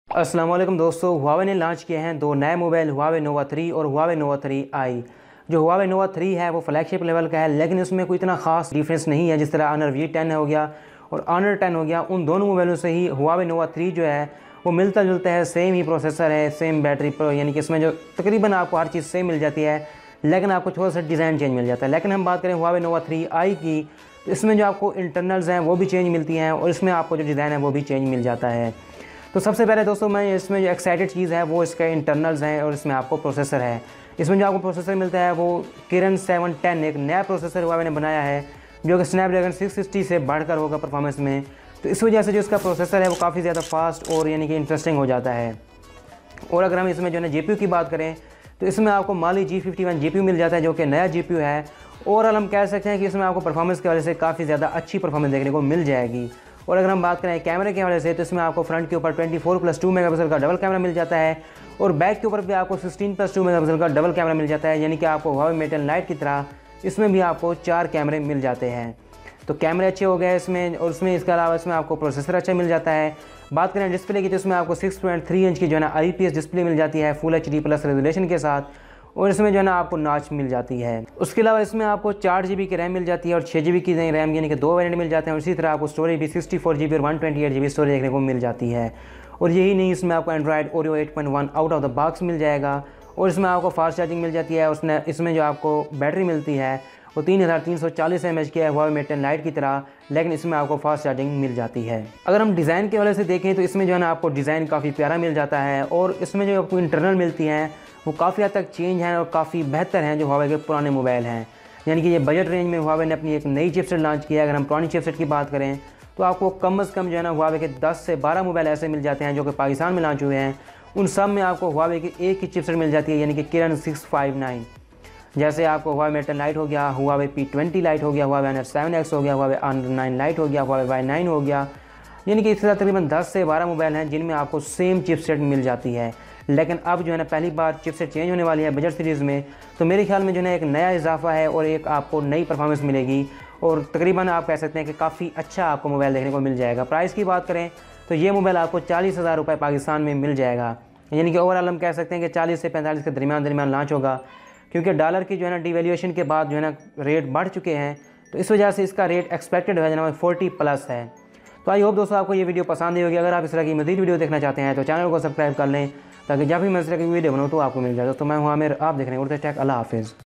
Huawei Nova 3i Full Review
Huawei Nova 3i Full Review - Pakistan
The Huawei Nova 3i is the most hyped up smartphone today and there is a big reason why that is so, check out the GPU turbo that boosted the Mali-G51 GPU which allowed the phone to play PUBG very smoothly.

The camera is ok, its very decent, its very similar to the nova 2i. It has AI now which is a great feature to have.